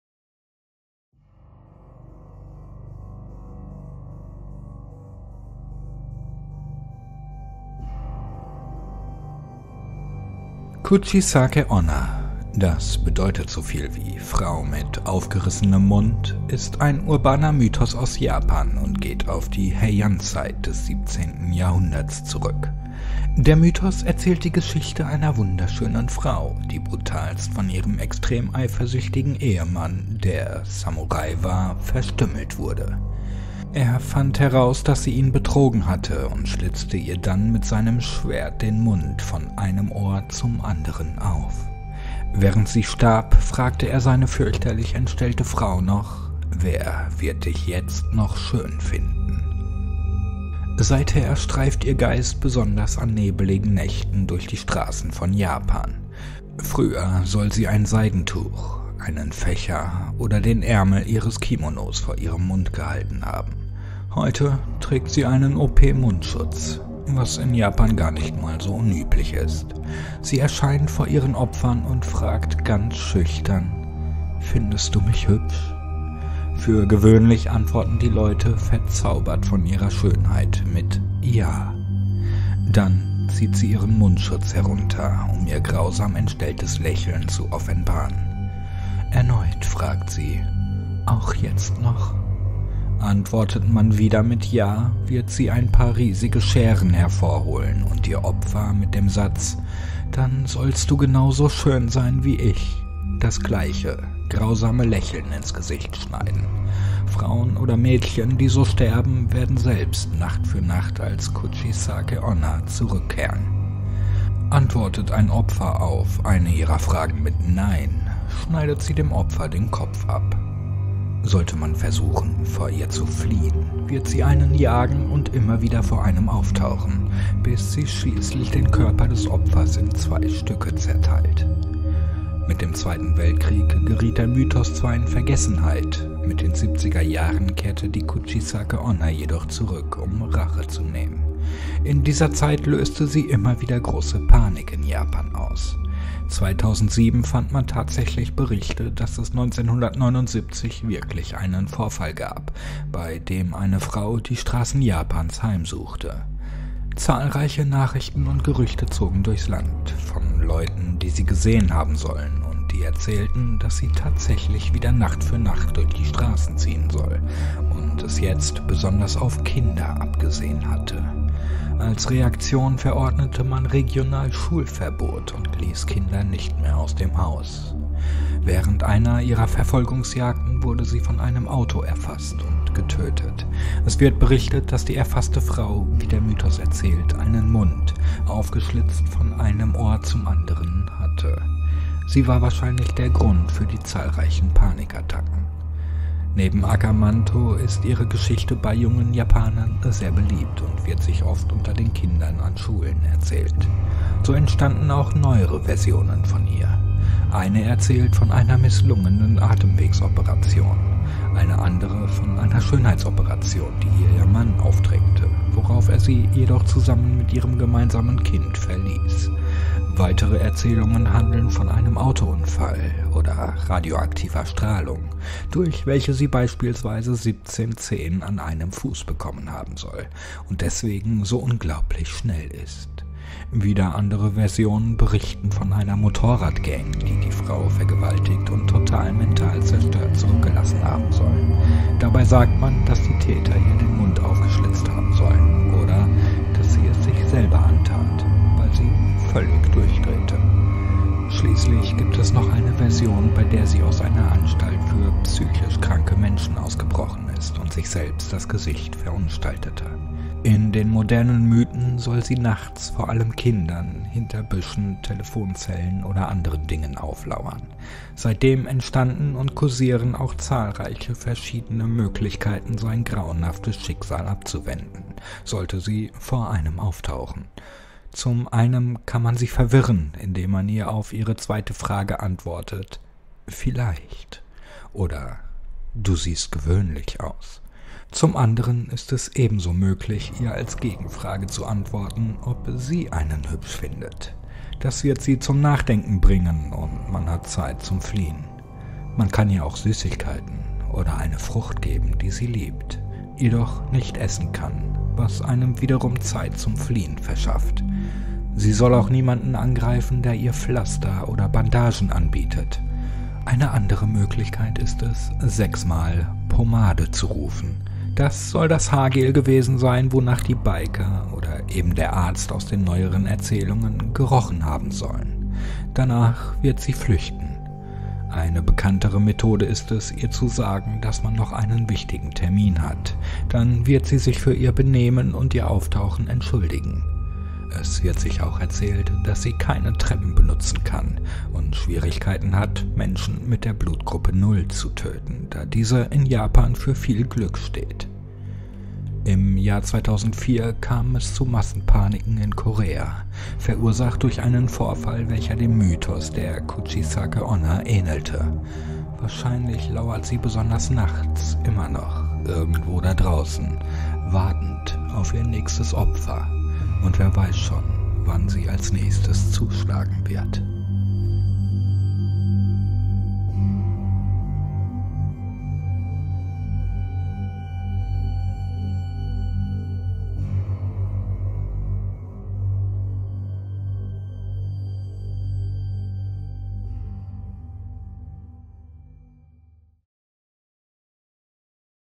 Kuchisake-Onna | Urbane Legende - Creepypasta German / Deutsch Horrorgeschichte ChrizzWhatever
Creepypasta "Kuchisake-Onna"

Habt Ihr eigene Geschichten, welche Ihr gerne von mir vertont haben möchtet?